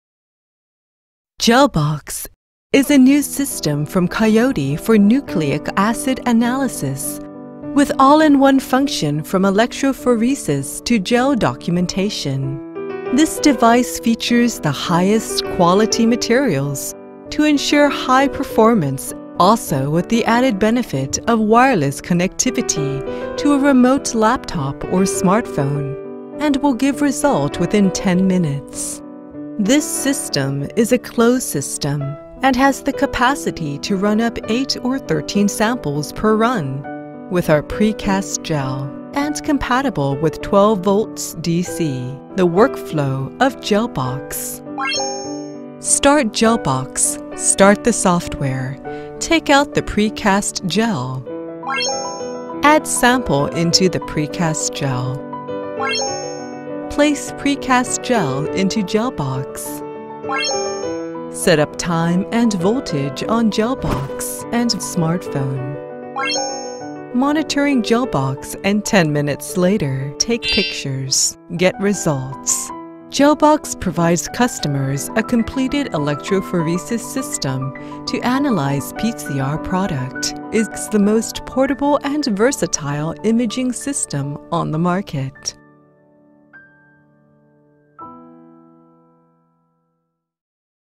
GelBox- All in one function electrophresis
GelBox is a new system from Coyote for nucleic acid analysis with all in one function from electrophoresis to gel documentation. This device features the highest quality materials to ensure high performance and reliability, also with the added benefit of wireless connectivity to a remote laptop or smartphone. And will give result within 10mins.

This system is a close system and has the capacity to run up 8 or 13 samples per run with our pre-cast gel.